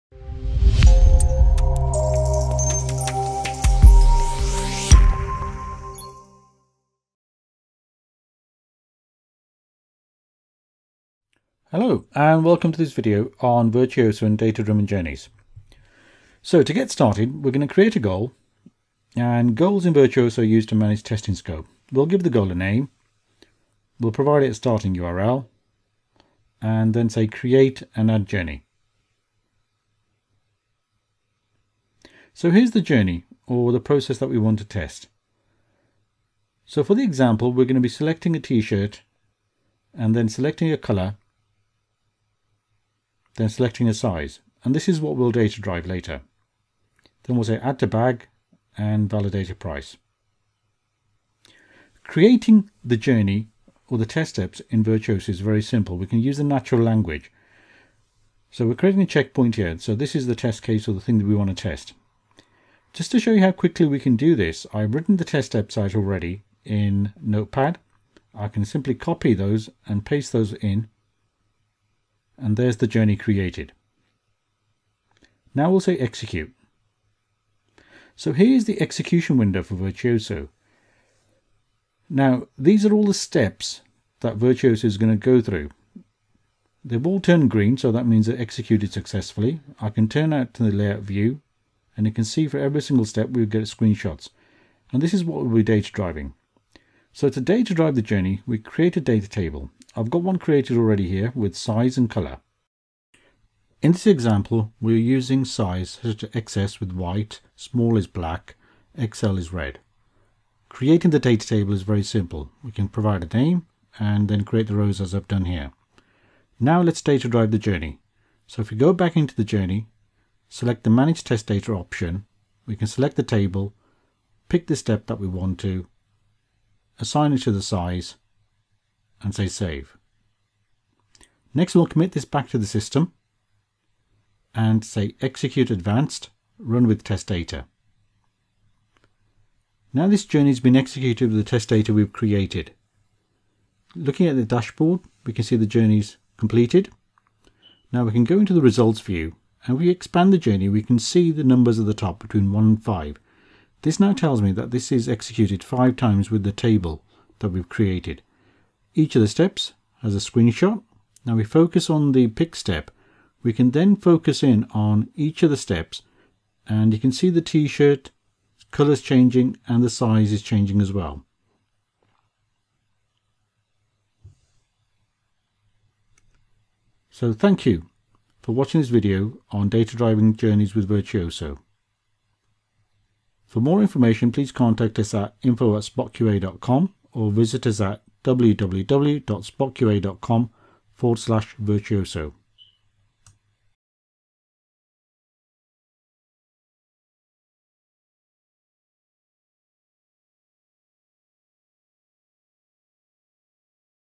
Data-Driven Tests Using Virtuoso's Test Data Management Feature
Learn how to create and use data tables and make your tests data-driven with ease.